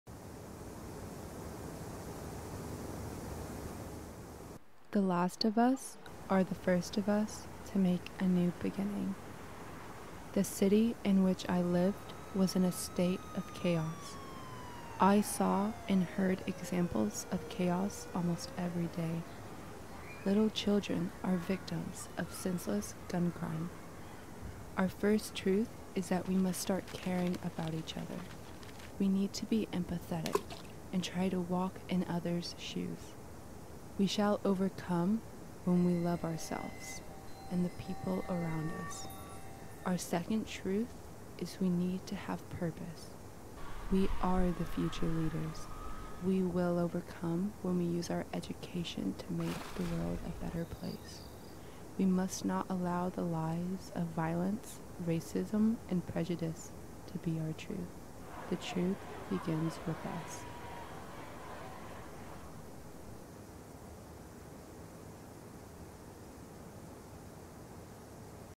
Ella Knott - Short Animation based on Sandra Parks
This is my short animation based on the award wining essay by Sandra Parks; a 13 year old girl from Milwaukee who was shot and killed by a stray bullet in her own home. Two years prior to her death she wrote an essay on gun violence, when she was 11 years old. The narration of this animation is taken from a passage of the essay.